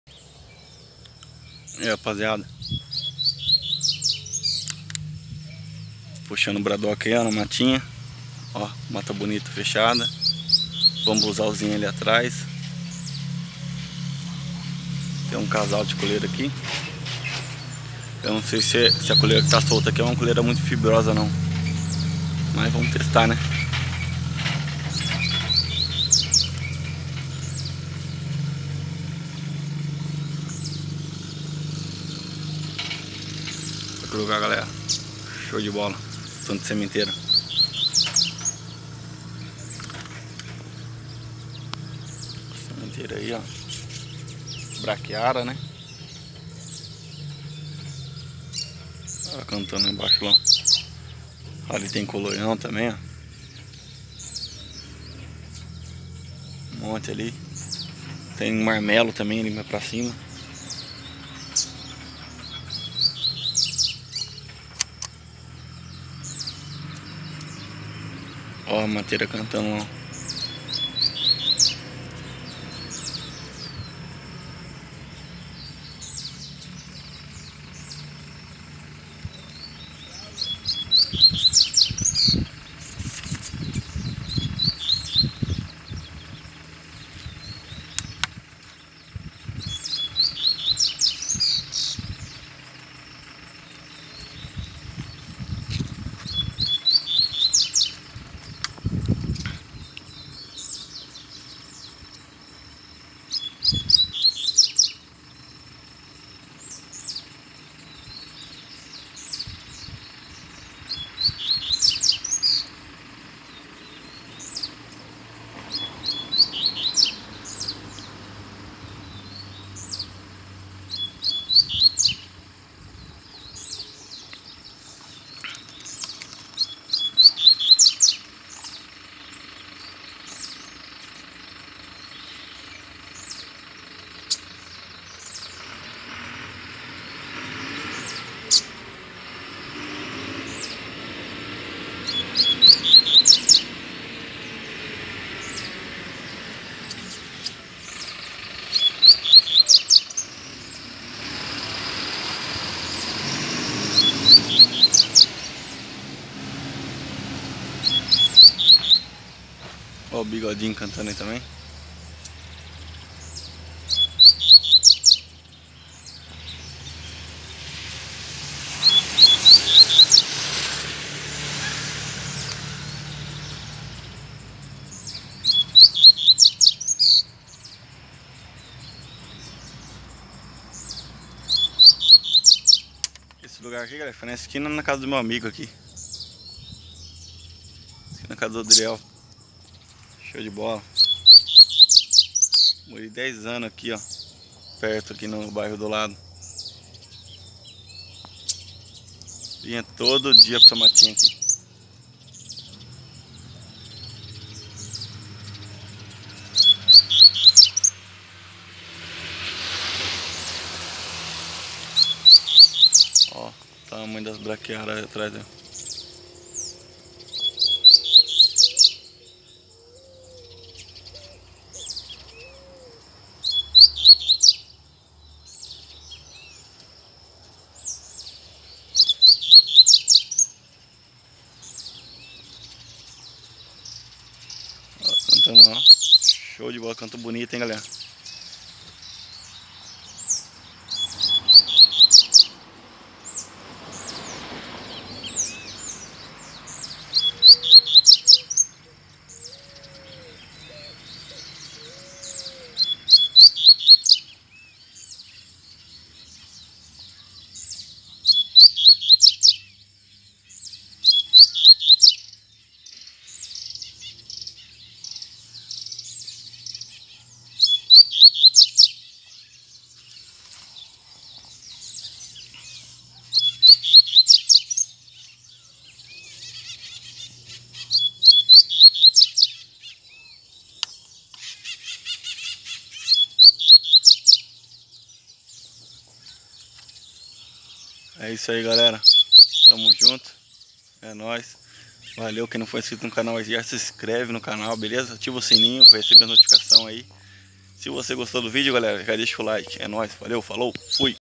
COLEIRO CANTAVA DE LONGE MAS NÃO VINHA NA GAIOLA POR NADA!!! - Coleiro Professor Aprendizado
Coleiro acasalando cantando muito mas não vem na Gaiola por nada!!!

Valeu passarinheiros !!!👏👍😃🐦